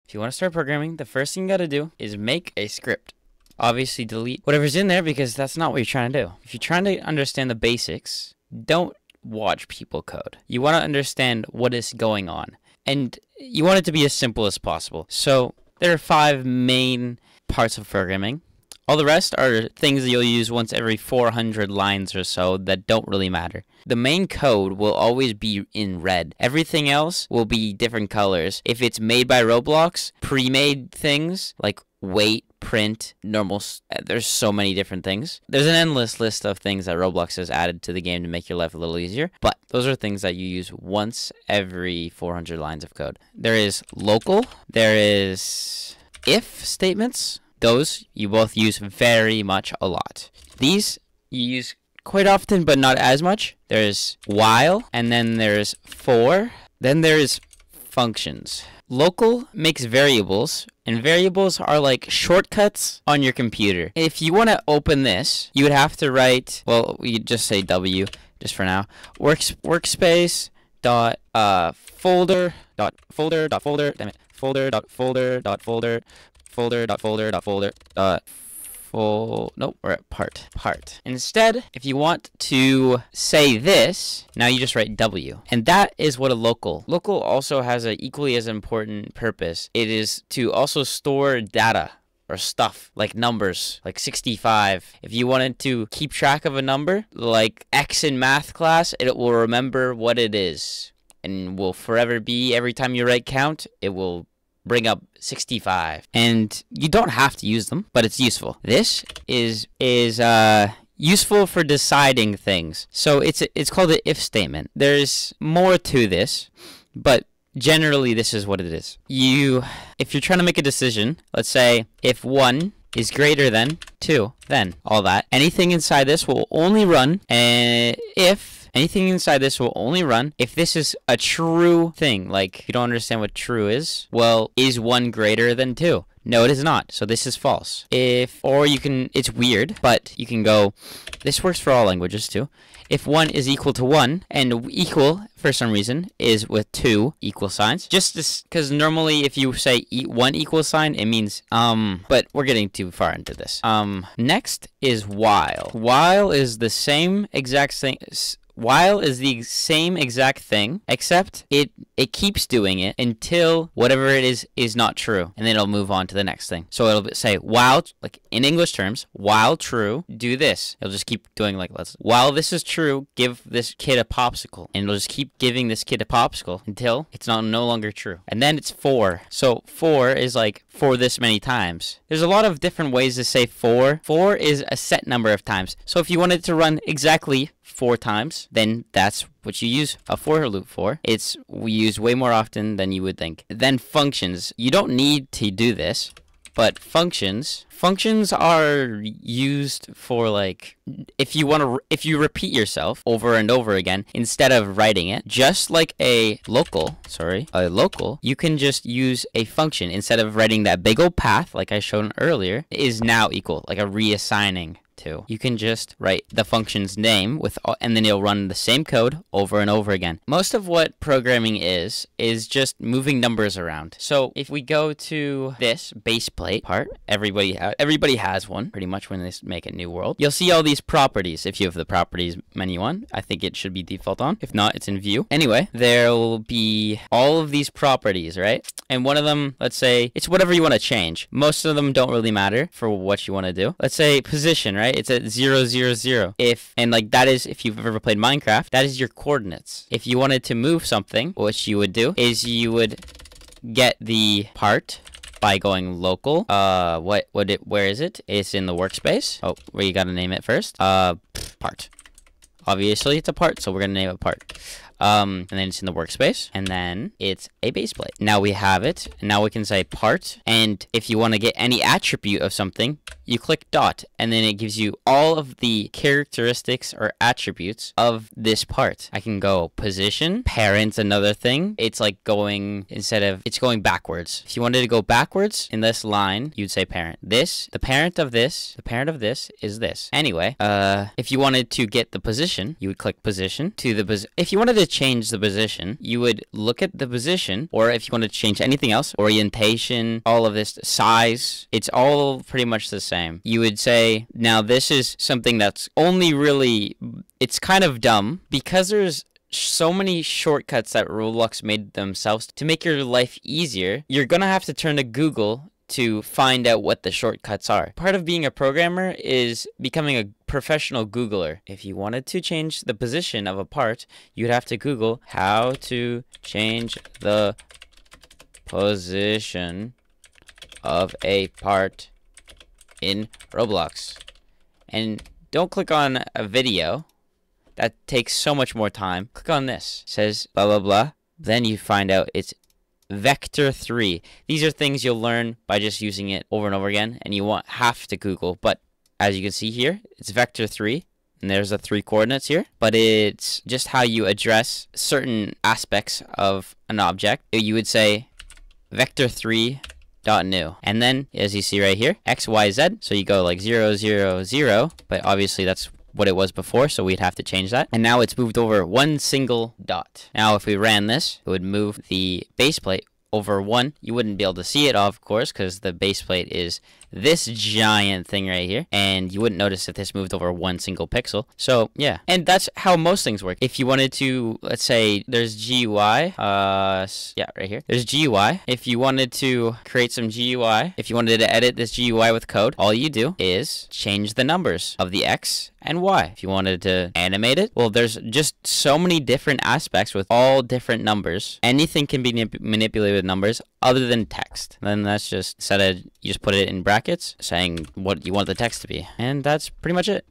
How to Start Scripting in 2025
This is a very beginner tutorial for people starting scripting with Lua U. This will teach you how to learn and what to learn when it comes to scripting in Roblox Studio in the best and easiest way. This will go over all of the basics. For 2025